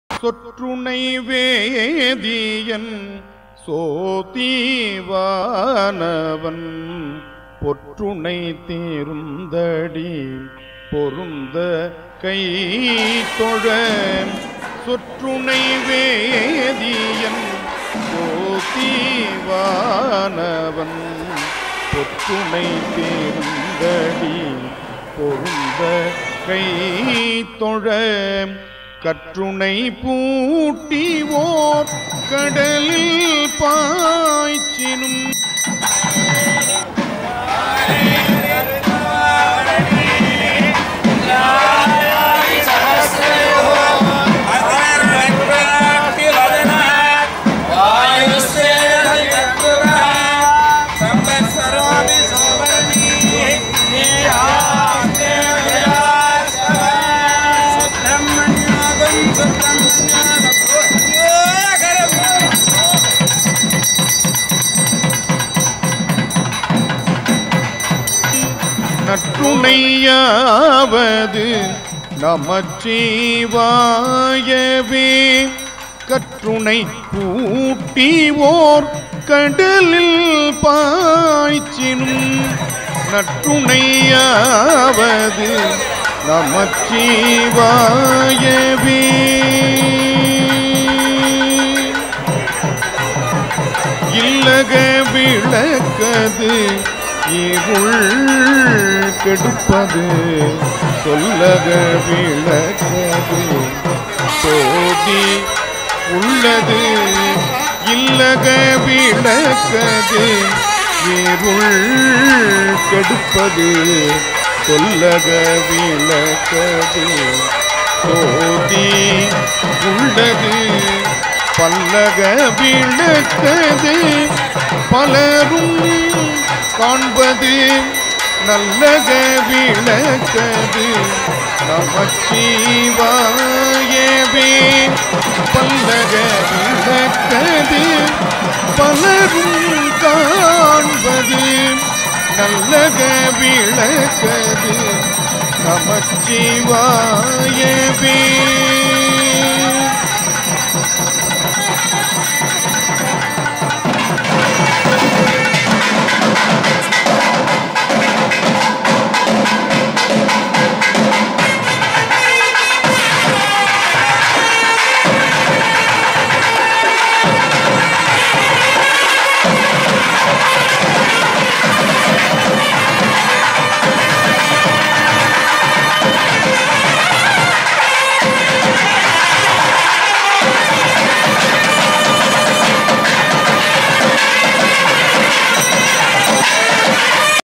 Chariot crown installation
This video shows the installation of the crown to the Sydney Murugan Chariot.